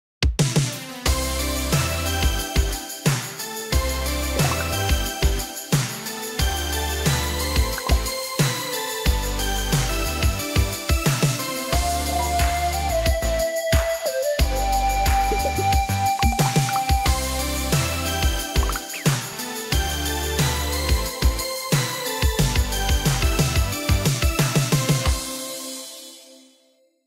FEAHLM45F 費雪學習棋盤遊戲
1. 動手活動， 促進小手肌肉發展。
2. 不同的積木形狀和顏色，滾動旋轉器和趣味機關，可以觸發學習內容和點亮小星星。
3. 在棋盤上移動棋子，並在過程中學習數字和顏色。
4. 智玩三階段，針對寶寶不同階段的成長，提供3個級別，由淺入深的學習內容。